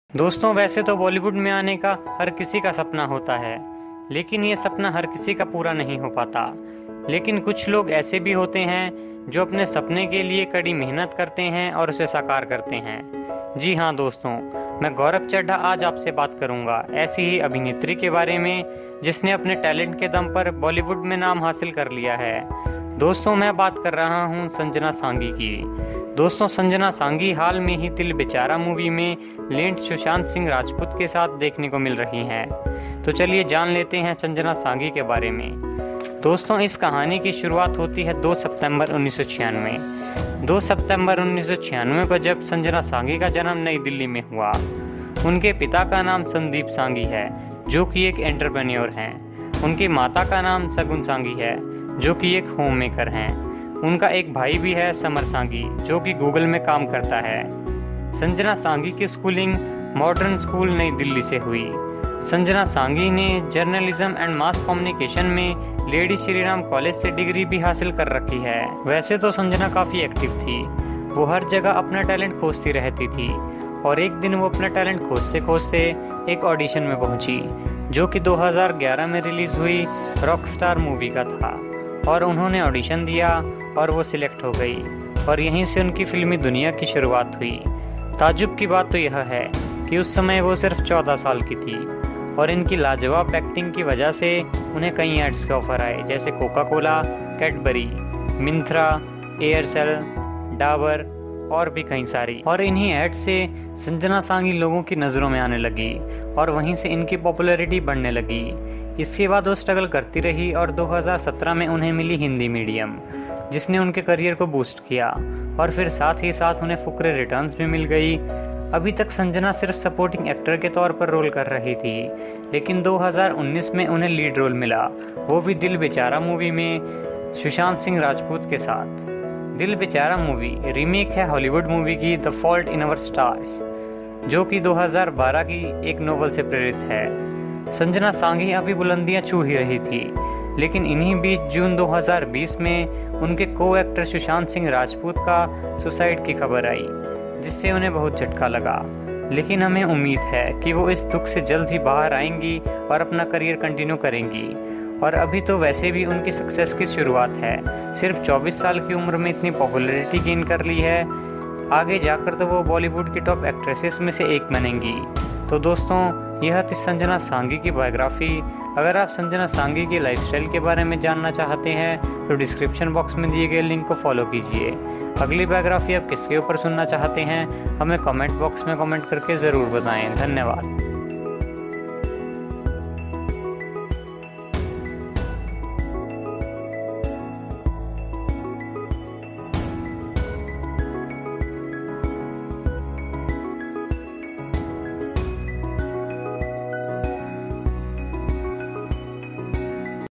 Sanjana Sanghi Biography | Dil Bechara Movie से आयी थी सुर्ख़ियों में - Struggle
This Video Is About Sanjana Sanghi Biography , the struggle of Sanjana Sanghi to become a star actress  .

This video is totally based upon my internet research .

Biography with GC
Sanjana Sanghi